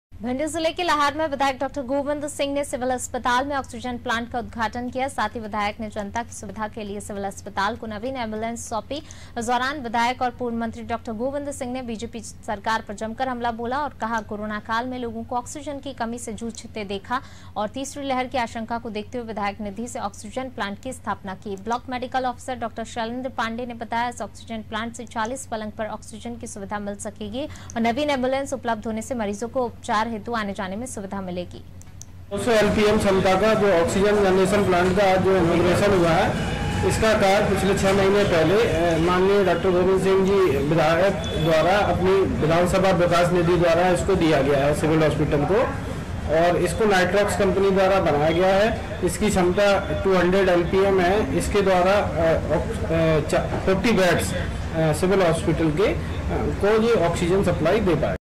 भिंड : लहार सिविल अस्पताल को सौगात | Aaj ki Taza Khabar | Anaadi Tv | Hindi News
भिंड जिले के लहार में विधायक डॉक्टर गोविंद सिंह ने सिविल अस्पताल में आक्सीजन प्लांट का उद्घाटन किया...इसके साथ ही विधायक ने जनता की सुविधा के लिए सिविल अस्पताल को नवीन एंबूलेंस सौंपी गई...इस दौरान विधायक और पूर्व मंत्री डॉक्टर गोविंद सिंह ने बीजेपी सरकार पर जमकर हमला बोला...और कहा कि कोरोना काल में लोगों को ऑक्सीजन की कमी से जूझते देखा...तो और तीसरी लहर की आशंका को देखते हुए विधायक निधि से आक्सीजन प्लांट की स्थापना की गई...ब्लाक मेडीकल आफिसर डॉक्टर शैलेंद्र पांडेय ने बताया कि इस आक्सीजन प्लांट से चालीस पलंग पर आक्सीजन की सुविधा मिल सकेगी...और नवीन एम्बुलेंस उपलब्ध होने से मरीजों को उपचार हेतु आने जाने में सुविधा मिलेगी...